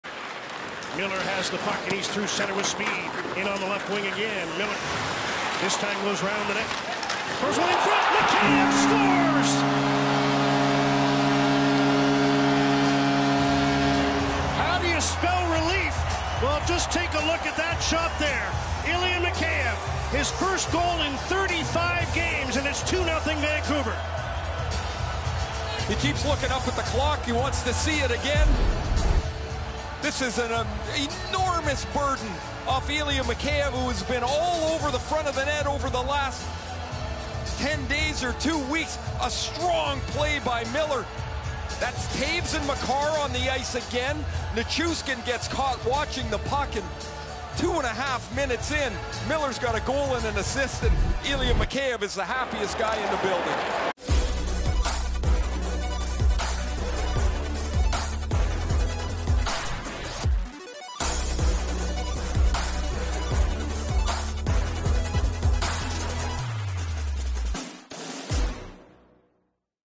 Canucks' Ilya Mikheyev Converts In Front To End 35-Game Goal Drought
Watch as Vancouver Canucks forward breathes a sigh of relief after converting in front against the Colorado Avalanche to end his 35-game goal drought.